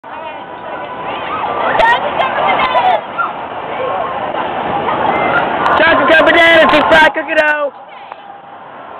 Girl In Banana Suit Does Cart-Wheel
This is a video of my friend Adrienne at the Fall Festival in evansville, Indina. We were trying to get people to buy chocolate covered bananas from her church's booth/stand thing. They happened  to have a banana suit on hand, and Adrienne decides to forget her social life for the next five months and wear it. And then I convinced her to do a cart-wheel and let me film it.